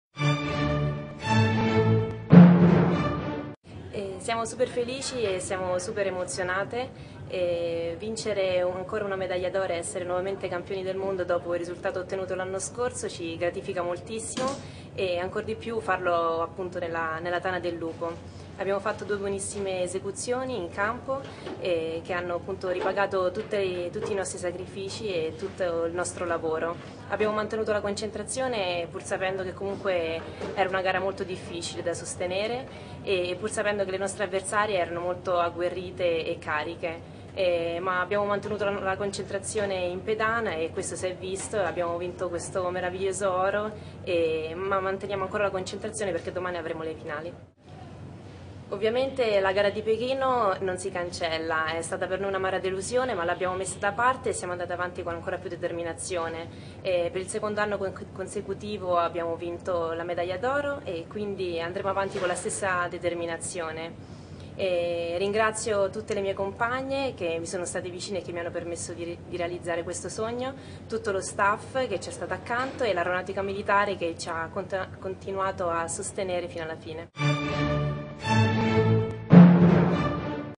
Rhythmic Gymnastics World Championships MOSCOW 2010 Elisa Santoni
30° Campionato del Mondo di Ginnastica Ritmica
VIDEO: Federazione Ginnastica d'Italia